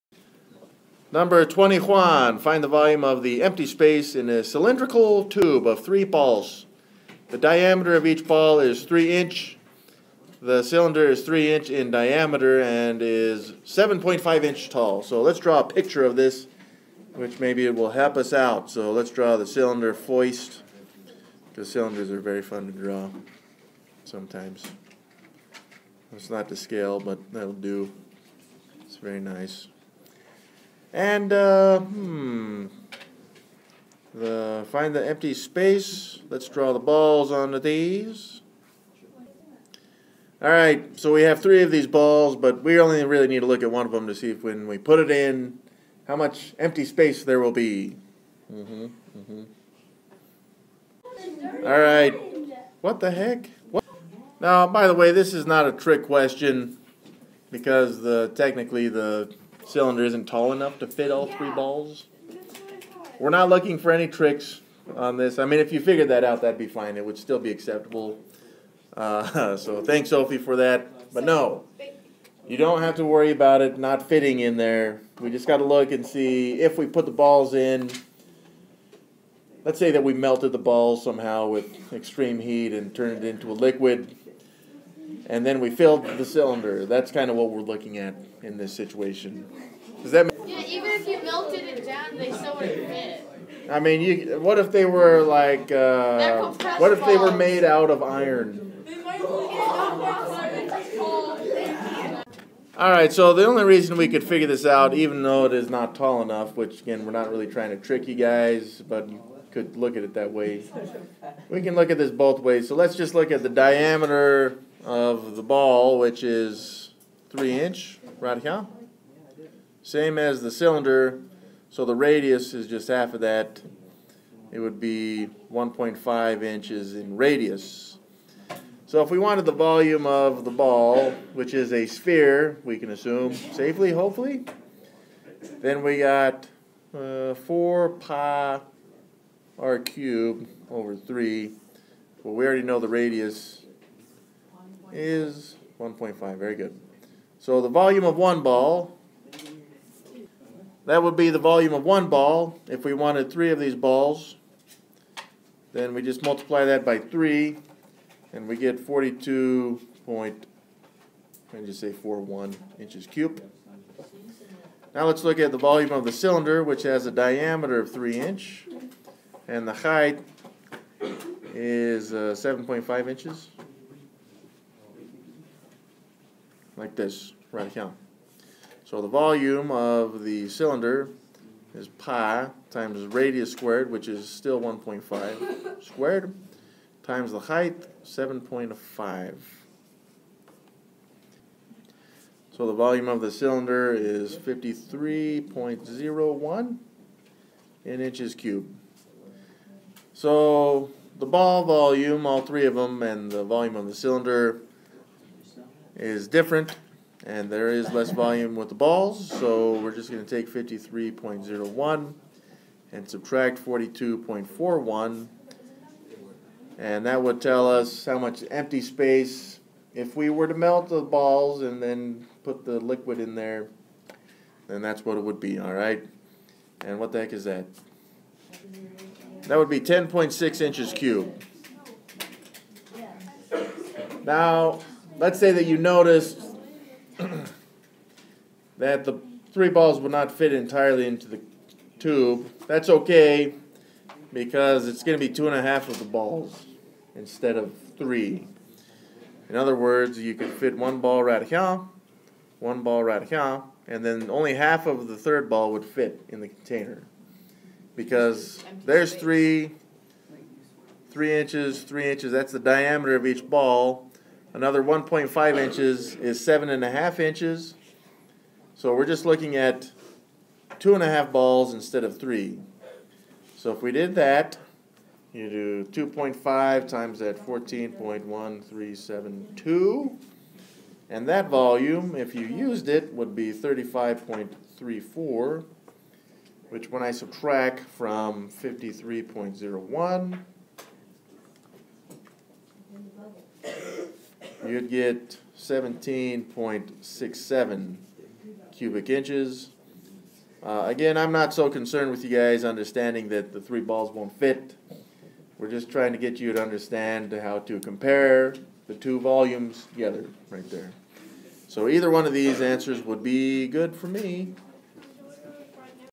How to Use Volume to Find Empty Space
The problem:
Find the volume of the empty space in a cylindrical tube of three balls.  The diameter of each ball is 3 inches.  The cylinder is 3 inches in diameter and is 7.5 inches tall.

The solution:
1. If all 3 balls fit: 10.62 cubic inches of empty space.
2. If only 2 and a half balls fit: about 1.67 cubic inches of empty space.